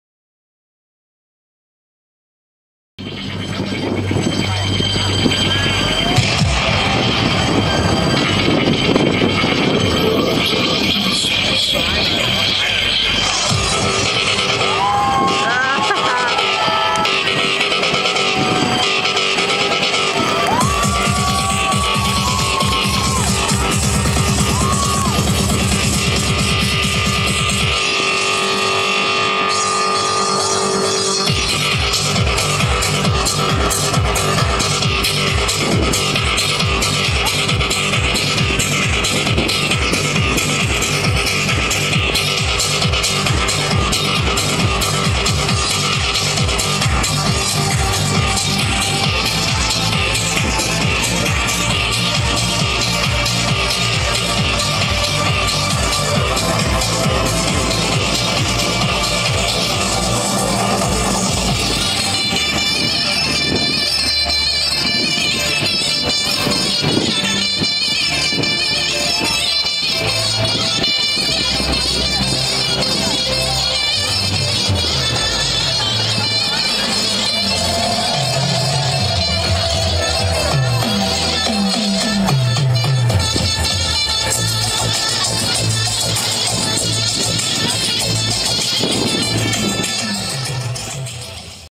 Fireshow in Koh Samet
Ploy Talay Restaurant in Koh Samet host some great nights with music and a fire show not to be missed. Please visit
to see our blob on Koh Samet.